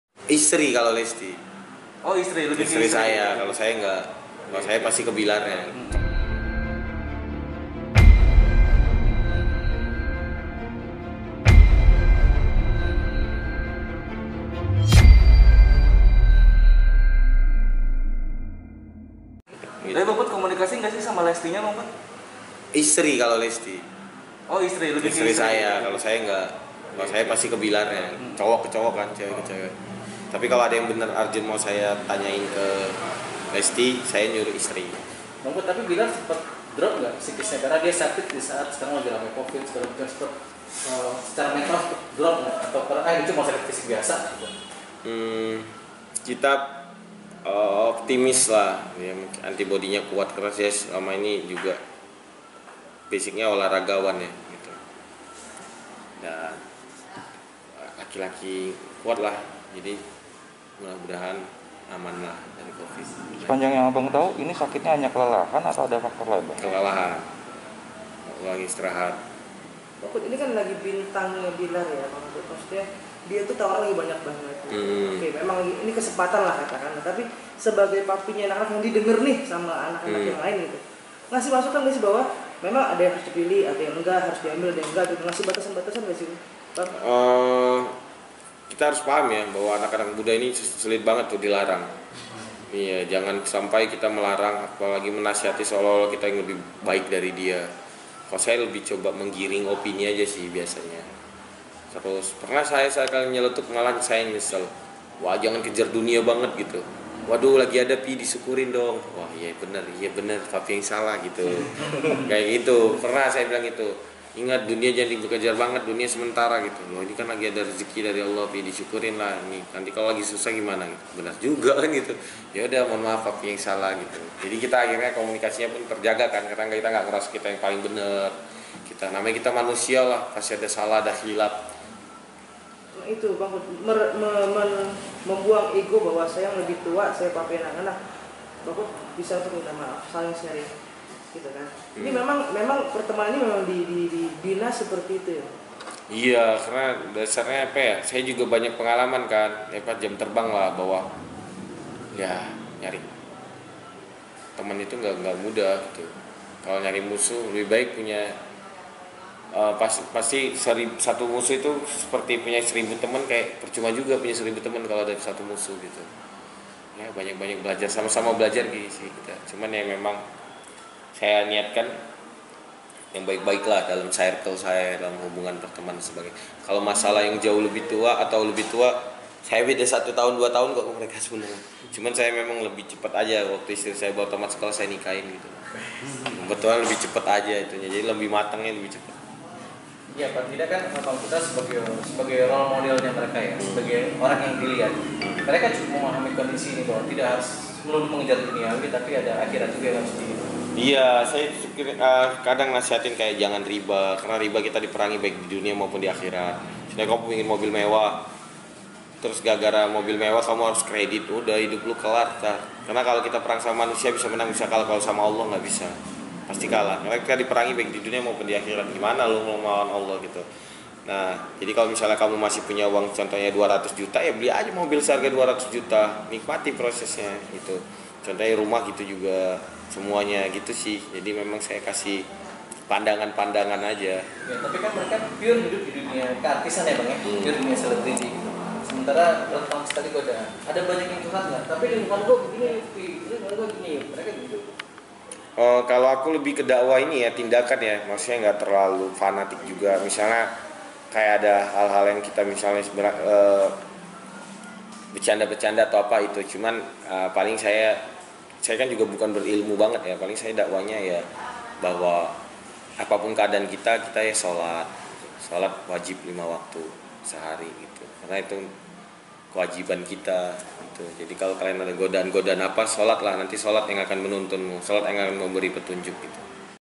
PESAN PUTRA SIREGAR UNTUK RIZKY BILLAR MENJAGA KONDISI KESEHATAN
Putra Siregar memiliki pesan kepada Rizky Billar agar menjaga kesehatan.